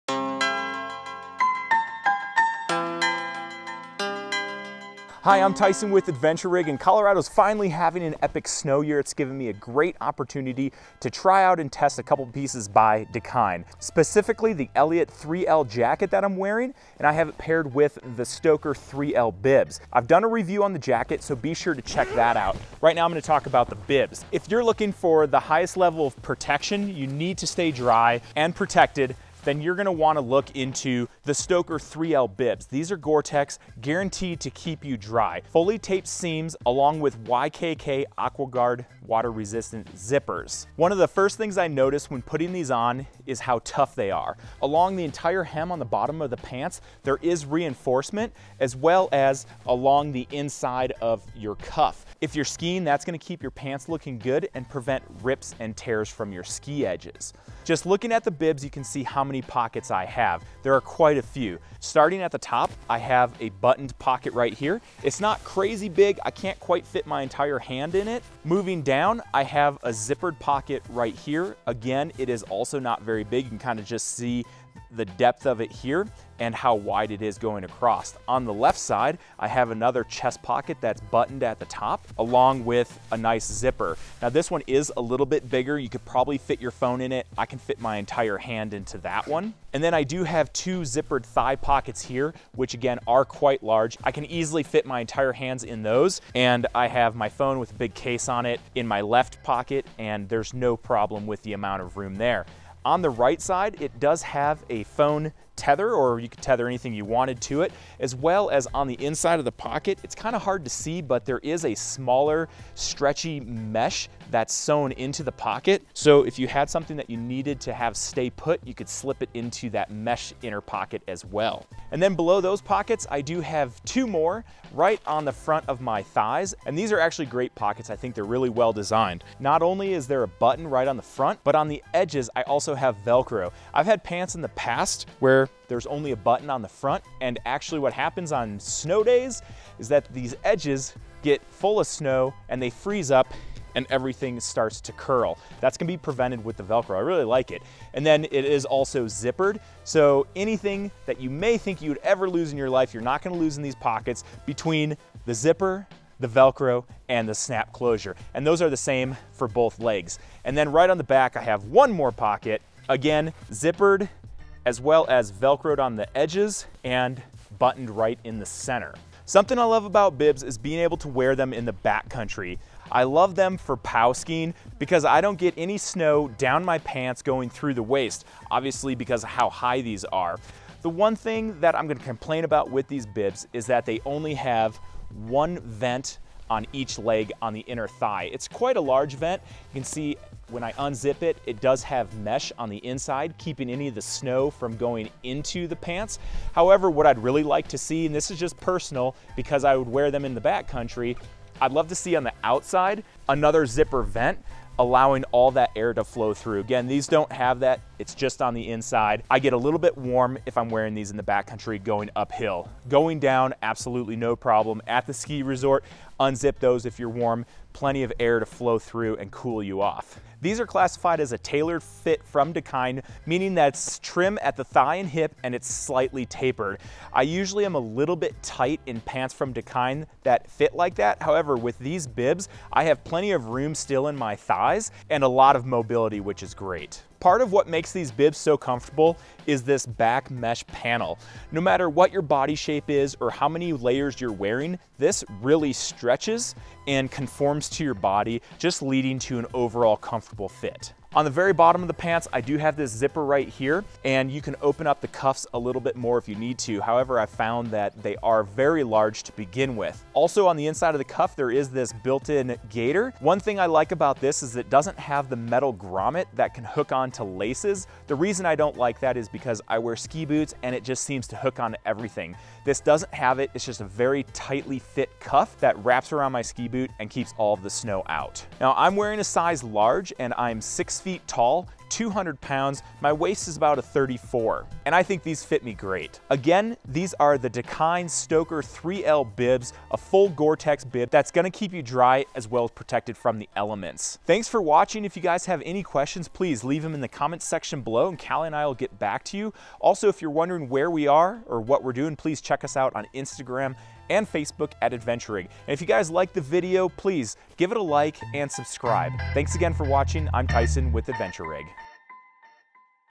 Stoker Gore-Tex 3L Bib by Dakine [Review]
Cally and I both picked up a pair of Gore-Tex Bibs by Dakine this year and we’re both equally obsessed. The Men's Stoker Gore-Tex 3L Bib by Dakine is comfortable, keeps me dry, and fits really well. My most favorite feature of the Stoker Bib is probably also the most obvious feature – they’re bibs.

Not only is Gore-Tex guaranteed to keep you dry, it’s also very breathable which is what makes these the ultimate backcountry shred pant. On the way up, your temp will still regulate and on the way down you won’t get drenched from knee deep pow. In addition to Gore-Tex these bibs are also built with YKK® AquaGuard water resistant zippers and fully taped seams. No matter what kind of elements you find yourself in, the Stoker Bib will keep you dry.

Sometimes I feel as though Dakine gear fits me a little snug but these fit great. I do like how Dakine has a consistent system of how they explain their fit system. These fall in the Tailored Fit category. I’m 200 pounds and 6’0 tall and the large is a perfect fit. They’re plenty long, not too tight in the thigh and I can still move freely on the mountain. I’m a tele-skier (but you probably already knew that. How do you know you’re at a party with a tele-skier? Oh they’ll let you know) so I need the ability to be able to move and bend without restriction.

The ample pockets throughout the bibs are another source of my love for these. There are a lot of pockets and they’re all usable. As well as many pockets, there’s also a Quick Fly hidden zipper so you don’t have to unzip your jacket or your bibs to go pee. It’s a simple feature really but it’s quite nice and can be fast and easy.

The Stoker Bibs have and will continue to take some abuse over the years and I’m confident that they’ll stand up to it. The material is durable and there is reinforcement where you need it. Especially since I’m droppin my knee, the knee reinforcement is so sweet. There is also reinforcement at the ankle and a strip of reinforcement all the way around the cuff. I have yet to mar these pants and that’s pretty impressive.

I tend to run warm and so I like the breathable mesh backing of the bibs. It allows for ample airflow and a nice fit no matter how many layers I have on. You can also unzip the inner thigh vents for airflow. One thing that I really would’ve loved to see is an outer leg zip for touring. That’s probably my only complaint with the Stoker bibs so really not too big of a deal.

Whether I’m skiing pow at the resort, touring in the backcountry, or riding snowmobiles, the Stoker Bib by Dakine takes abuse like a boss and keeps me dry and looking fly. I have these paired with the Eliot Jacket by Dakine so be sure and check out that review as well!

If you're interested in buying yourself some Stoker Bibs, using the provided link supports us in the fact that we get a small kickback from your Amazon purchase. We appreciate your support.

Links

Stoker Bib